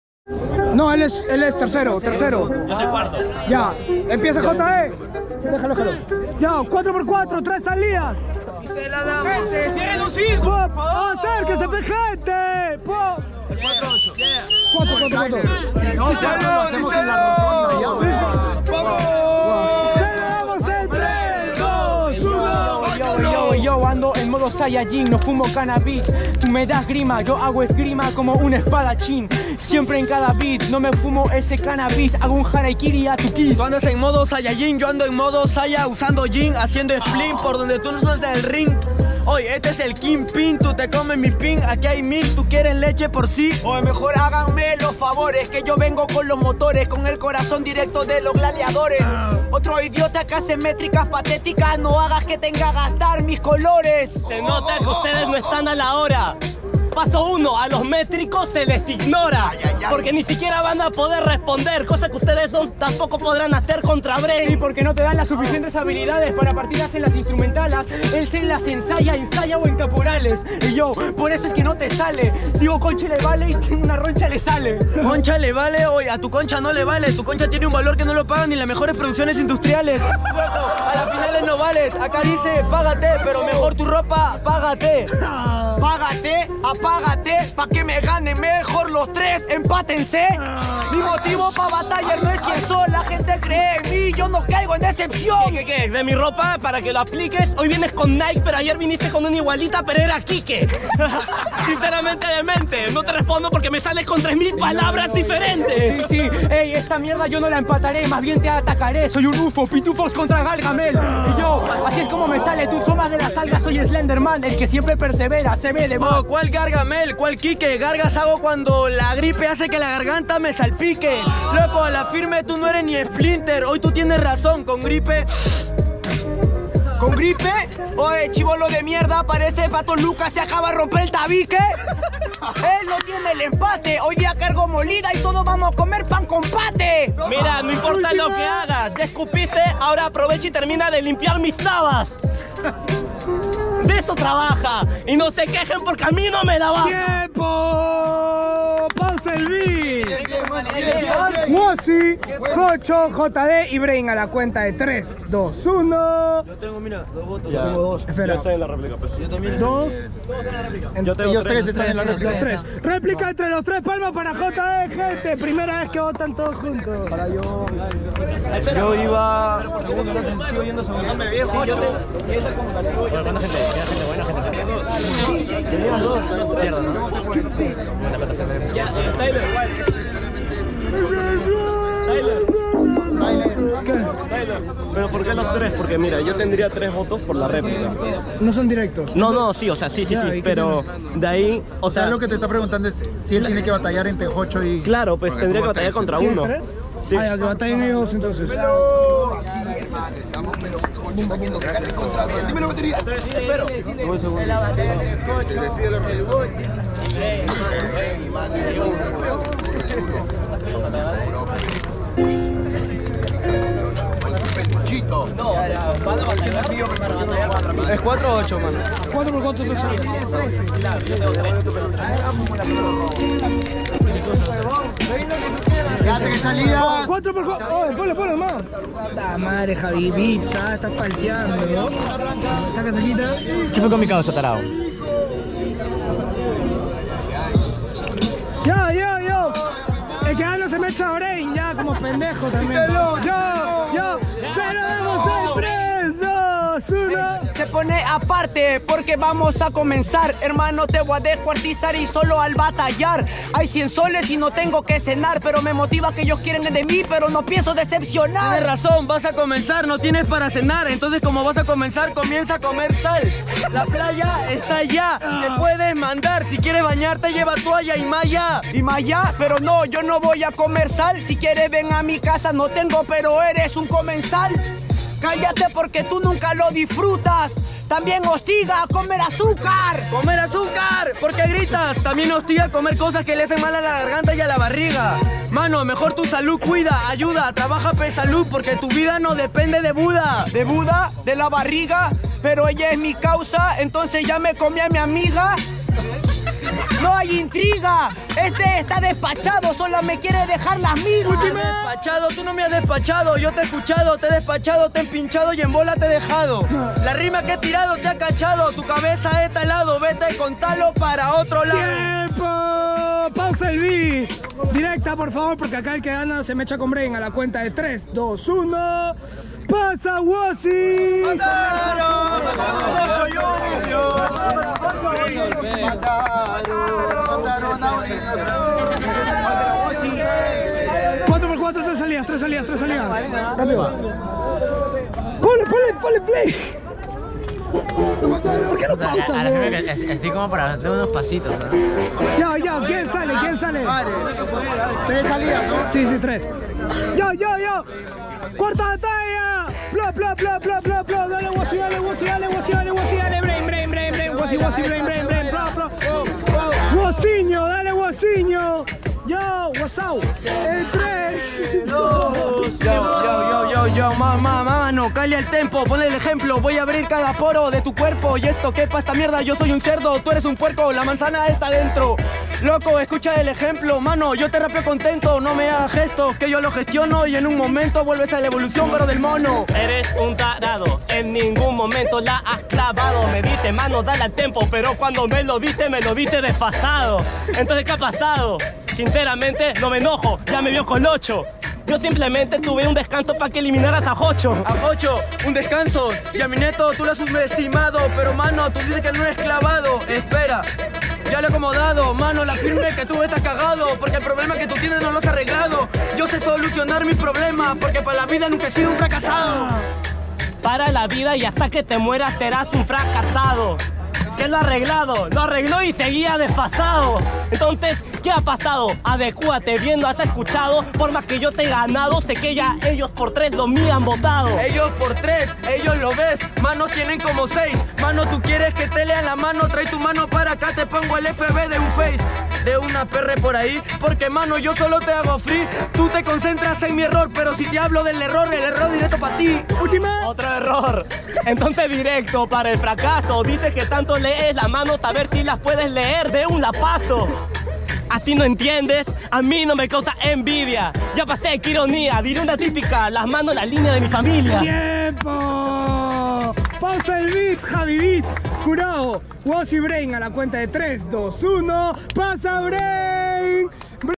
BRAIN vs JOCHO vs JD vs WOZI - OCTAVOS - LZ FINAL TOUR
Batalla entre Brain, Jocho, JD y Alexunder por la fecha de LZ Final Tour.